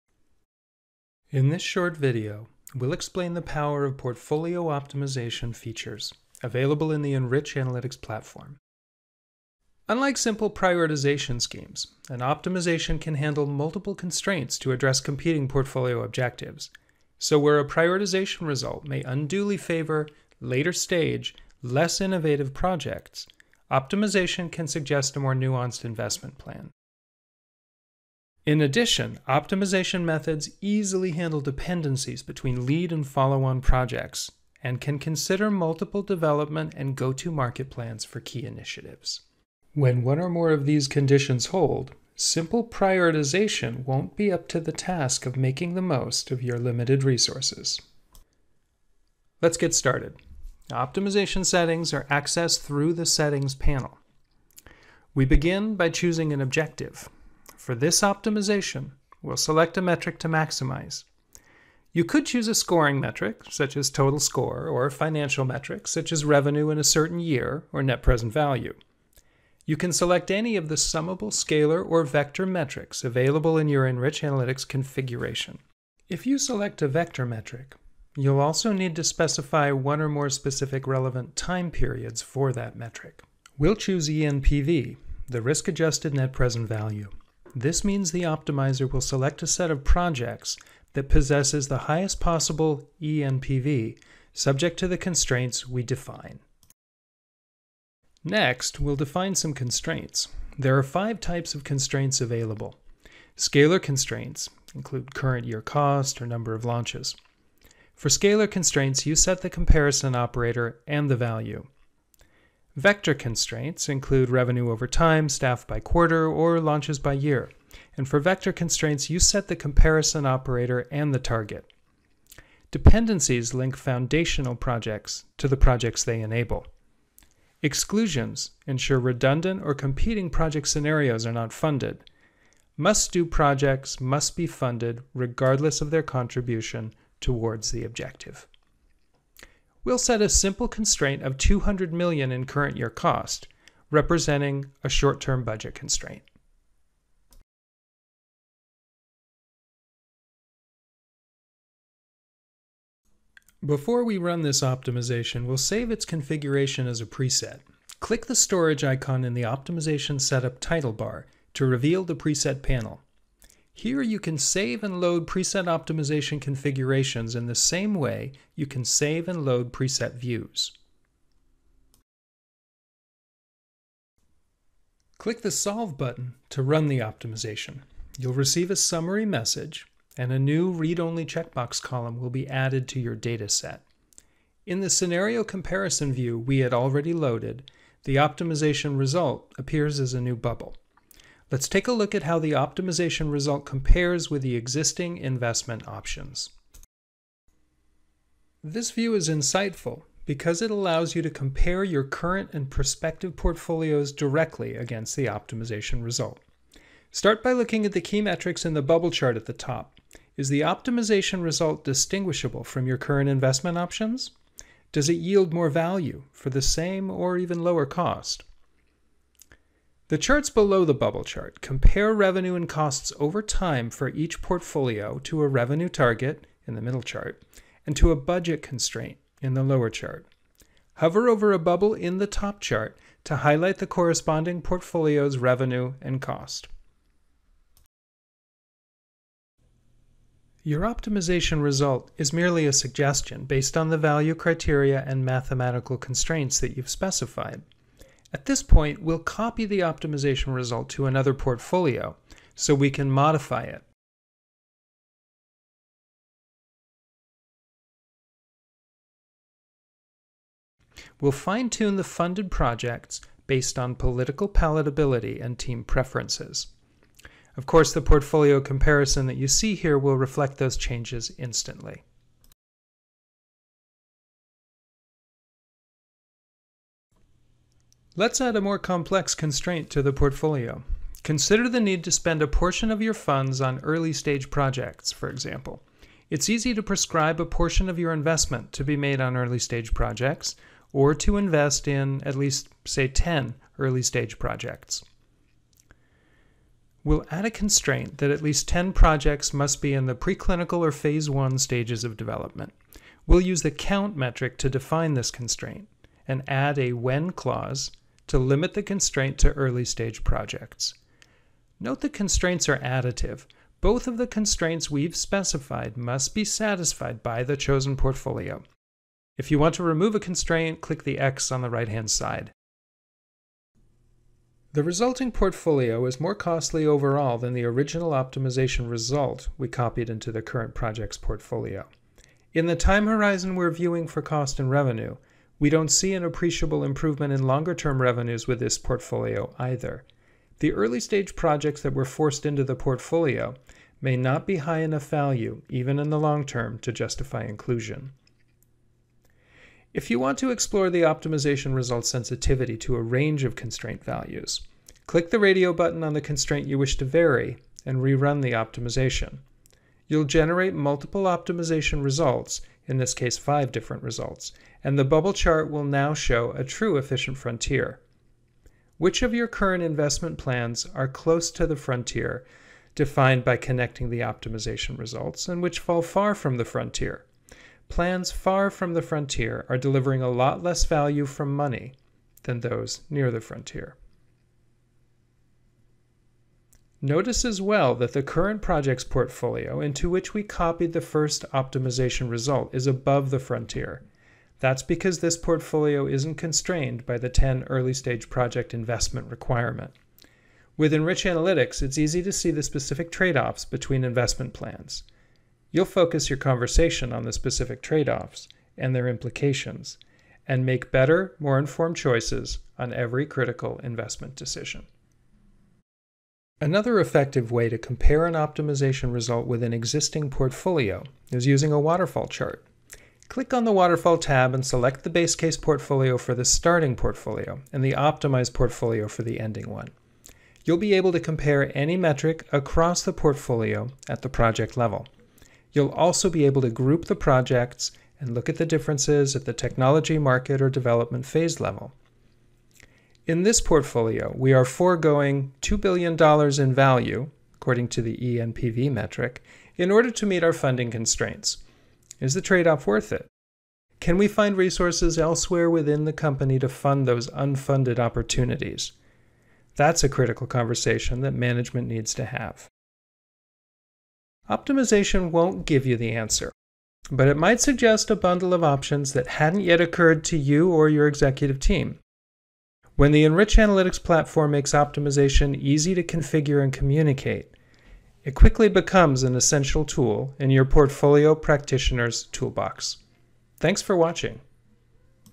Portfolio Optimization with Enrich Analytics
This video demonstrates portfolio optimization methods using the Enrich Analytics Platform.

Optimization is useful when:

 You need to balance short and long term portfolio objectives, or balance your investments across technologies, markets, or regions.
 Project cost varies dramatically across your portfolio of projects
 You have projects that are mutually exclusive
 You have projects that must be funded (perhaps for contractual or regulatory reasons)
 You have projects that depend on related projects also being funded
You have multiple development or market plans to choose from for some projects